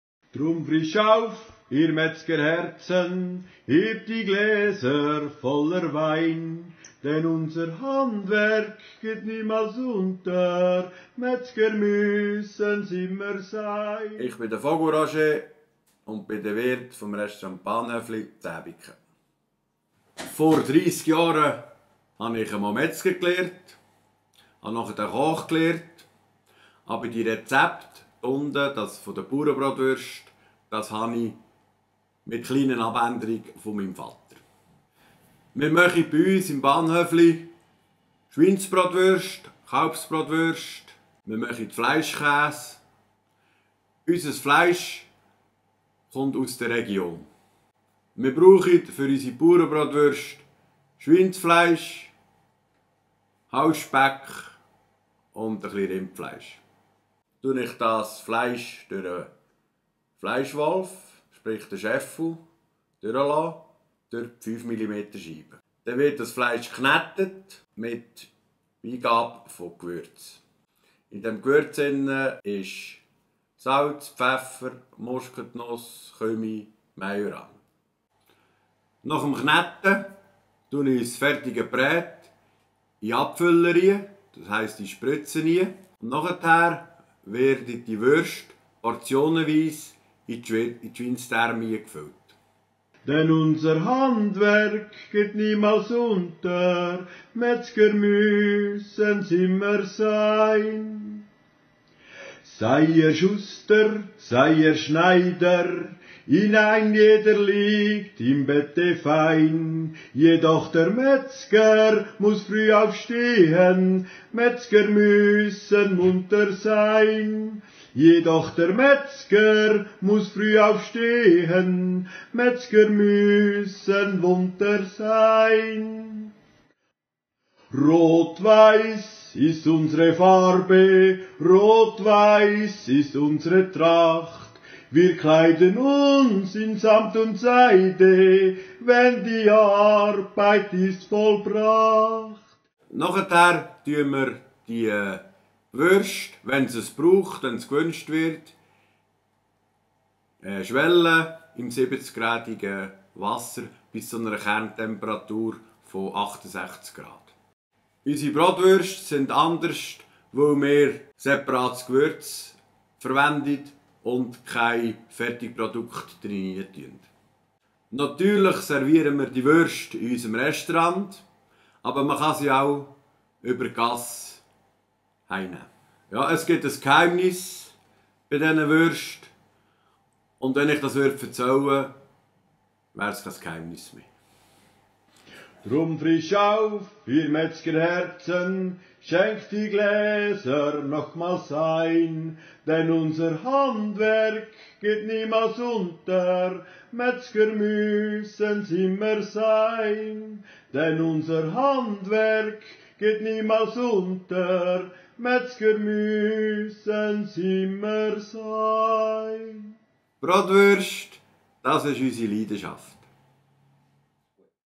Das Geheimnis der perfekten Bratwurst
Wie macht der "Wurstkönig" Roger Vogel vom Restaurant Bahnhöfli in Ebikon (LU) seine perfekten Bratwürste? Hier gibt es das exklusive Interview und einen Blick hinter die Kulissen.